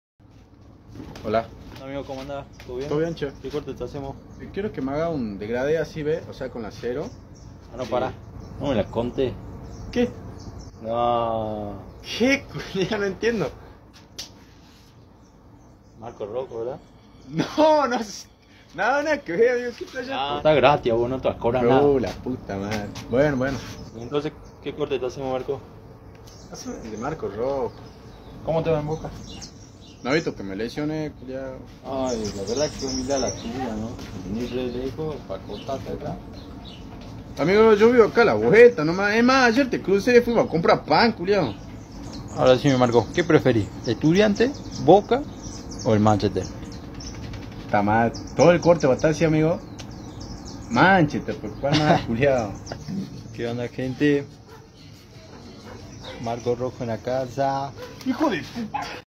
Marcos Rojo es humilde
Marcos Rojo y su humildad detras del futbol😲
Voy a mandar saludos a quien me siga y me mande md en ig📸